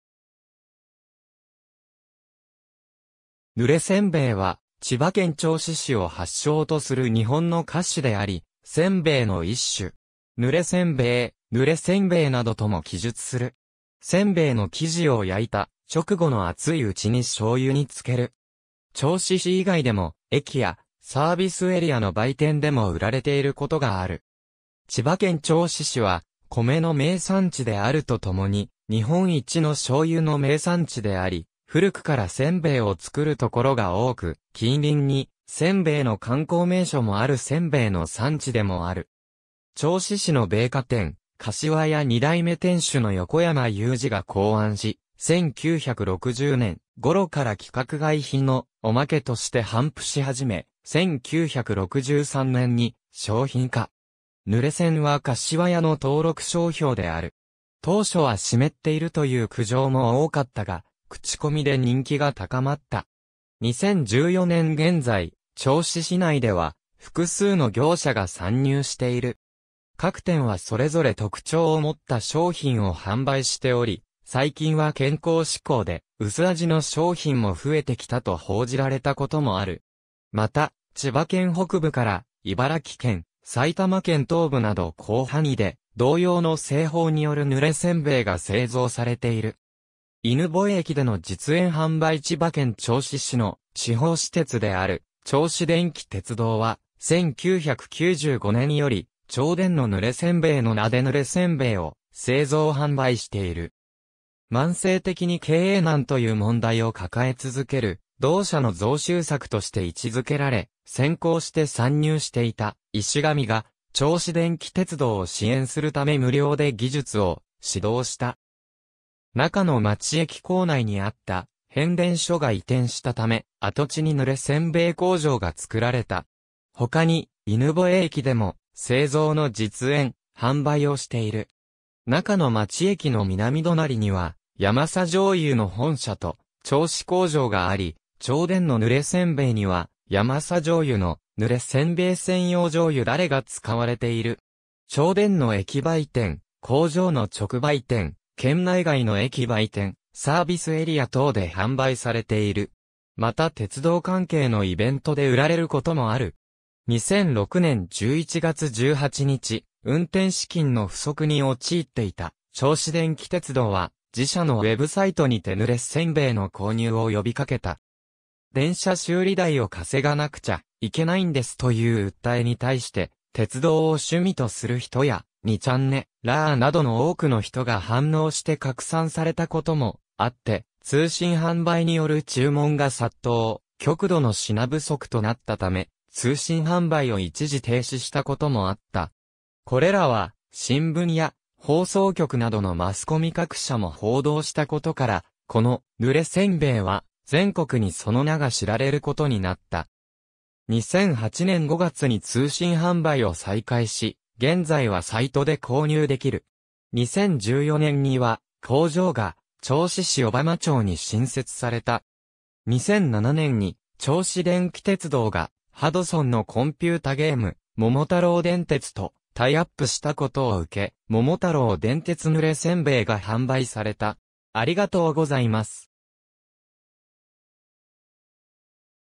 ぬれせんべい
ぬれせんべいは、千葉県銚子市を発祥とする日本の菓子であり、煎餅の一種。
ぬれ煎餅、濡れ煎餅などとも記述する。
煎餅の生地を焼いた直後の熱いうちに醤油に漬ける。
銚子市以外でも駅やサービスエリアの売店でも売られている事がある。
千葉県銚子市は、米の名産地であると共に日本一の醤油の名産地であり、古くから煎餅を作るところが多く、近隣に煎餅の観光名所もある煎餅の産地でもある。
銚子市の米菓店「柏屋」2代目店主の横山雄次が考案し、1960年ごろから規格外品の「おまけ」として頒布し始め、1963年に商品化。
「ぬれせん」は柏屋の登録商標である。
当初は湿っているという苦情も多かったが、口コミで人気が高まった。
2014年現在、銚子市内では複数の業者が参入している。
各店はそれぞれ特徴を持った商品を販売しており、最近は健康志向で薄味の商品も増えてきたと報じられたこともある。
また千葉県北部から茨城県、埼玉県東部など広範囲で同様の製法によるぬれ煎餅が製造されている。
犬吠駅での実演販売 千葉県銚子市の地方私鉄である銚子電気鉄道は、1995年より「銚電のぬれ煎餅」（登録商標）の名でぬれせんべいを製造販売している。
慢性的に経営難という問題を抱え続ける同社の増収策として位置づけられ、先行して参入していた「イシガミ」が銚子電気鉄道を支援するため無料で技術を指導した。
仲ノ町駅構内にあった変電所が移転したため、跡地にぬれ煎餅工場が作られた。
ほかに犬吠駅でも製造の実演・販売をしている。
仲ノ町駅の南隣にはヤマサ醤油の本社と銚子工場があり、銚電のぬれ煎餅にはヤマサ醤油の「ぬれ煎餅専用醤油だれ」が使われている。
銚電の駅売店・工場の直売店、県内外の駅売店・サービスエリア等で販売されている。
また鉄道関係のイベントで売られることもある。
2006年11月18日、運転資金の不足に陥っていた銚子電気鉄道は、自社のウェブサイトにてぬれ煎餅の購入を呼びかけた。
「電車修理代を稼がなくちゃ、いけないんです」という訴えに対して、鉄道を趣味とする人や「2ちゃんねらー」などの多くの人が反応して拡散されたこともあって通信販売による注文が殺到、極度の品不足となったため、通信販売を一時停止したこともあった。
これらは、新聞や放送局などのマスコミ各社も報道した事から、この『ぬれせんべい』は全国にその名が知られることになった。
2008年5月に通信販売を再開し、現在はサイトで購入できる。
2014年には工場が銚子市小浜町に新設された。
2007年に銚子電気鉄道がハドソンのコンピュータゲーム『桃太郎電鉄』とタイアップしたことを受け「桃太郎電鉄 ぬれ煎餅」が販売された。